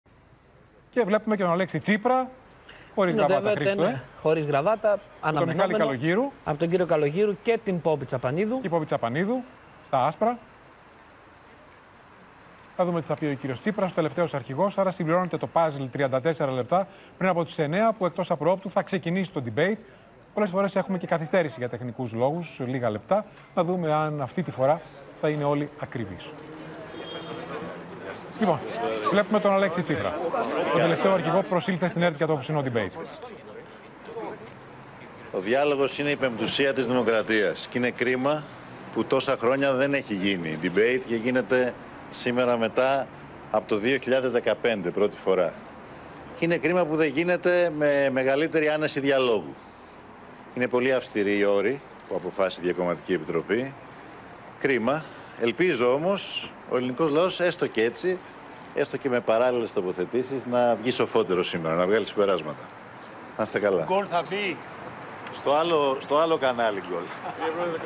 Η άφιξη του Αλέξη Τσίπρα στο Ραδιομέγαρο της ΕΡΤ | OPEN TV
Αυστηροί οι όροι της σημερινής τηλεμαχίας, κρίμα, είπε χαρακτηριστικά ο Αλέξης Τσίπρας κατά την άφιξη του στο Ραδιομέγαρο της ΕΡΤ.